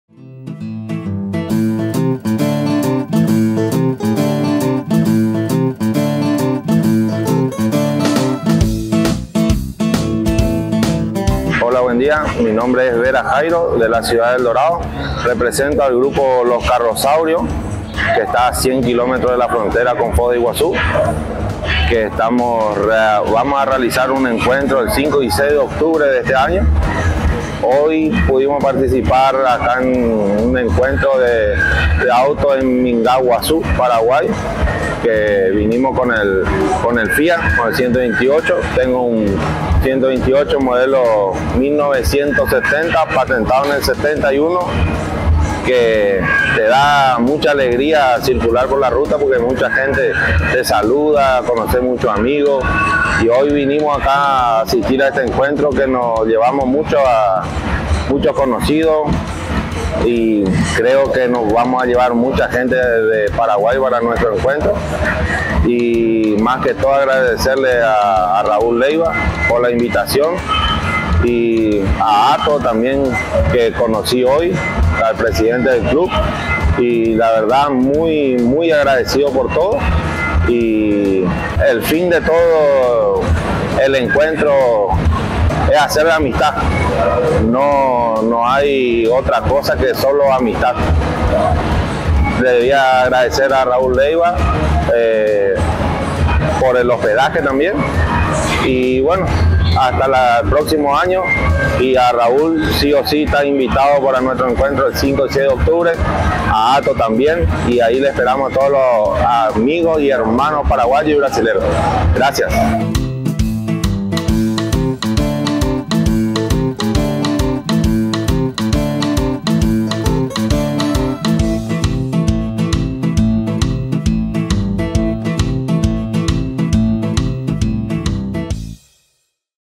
Invitación 10mo Encuentro Internac de vehículos antiguos, clásicos y especiales Carrosaurios 2024
Evento a realizarse los días 5 y 6 de octubre 2024 en la ciudad de El Dorado Misiones Argentina.
Organiza : La Agrupación de Autos Antiguos, Clásicos y Especiales Los Carrosaurios.